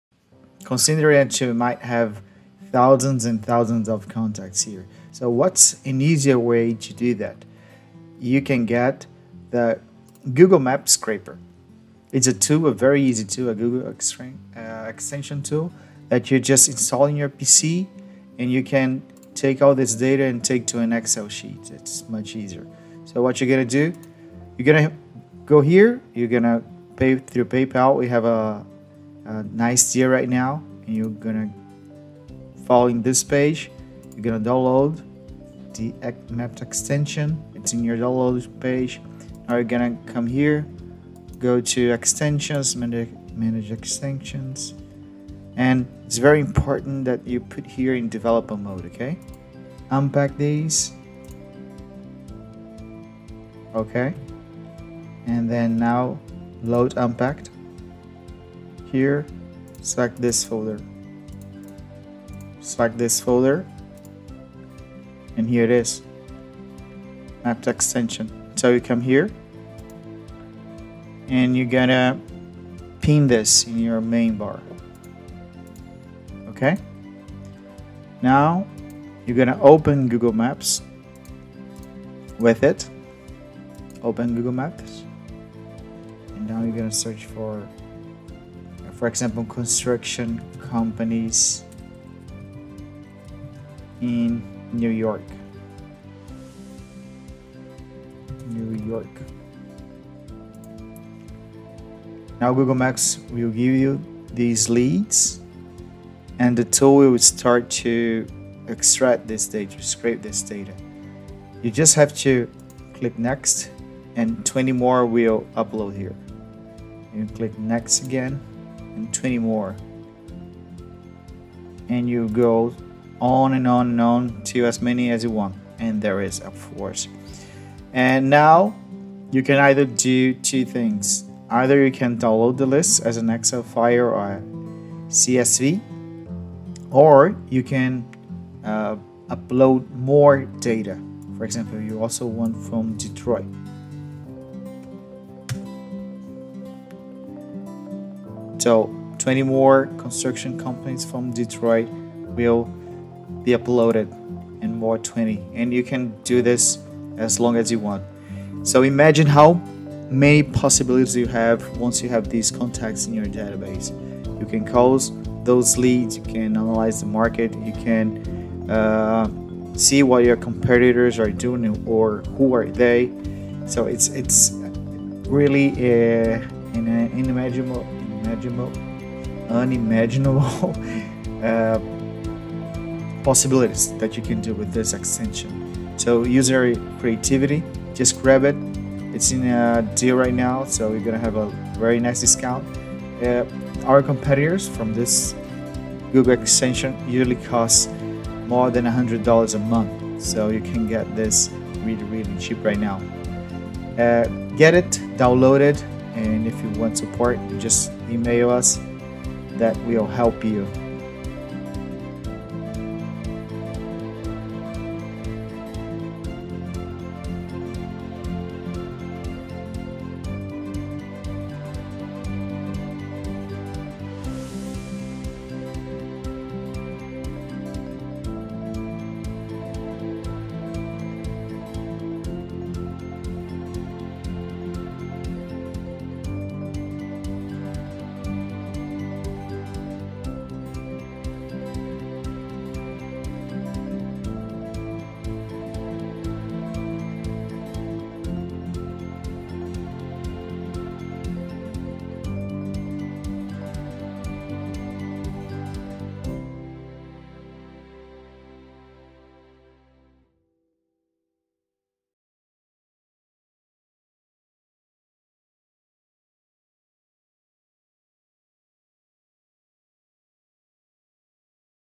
Google Maps Import CSV - [Fast and Easy] Export Data Google Maps | Google Maps Scraper
In this video, you will know how to extract data from google maps to excel. Extract data from google maps to excel is now very easy.
You just have to install Chrome Extension to you browser and extract data from google maps api with one simple click.
You will be exporting data from google maps and download it to excel as xls or cvs file.
You also have full name, website, telephone number, ratings and any other information publicly registered in Google Maps.
Pull data from google maps just updating the page. It will get 20 results at a time. Just click “next” and 20 more will load.
Download the file and Voilá! Your data from google maps in an excel file.

Ready to extract all data from businesses in Google Maps with a couple of clicks to an excel file?

It is very simple to use!

Install the Map Scraper on your Google Chrome.

Open Google Maps and search for the type of businesses you are looking for, such as "restaurants near Manhattan".

The extension will automatically load the results on the page. Click Next to the following 20 and so on.

When you are ready, download the file in excel or .cvs and filter the data. That way you can google maps import cvs.

Start calling your new prospects and close more sales.

Or maybe you just want to do a market research on your competitors

Maps Scraper will save you tons of time and have you achieve your goals.

Filter the data of any type of business anywhere in the world.

It will be in your hands within minutes.
Good businesses!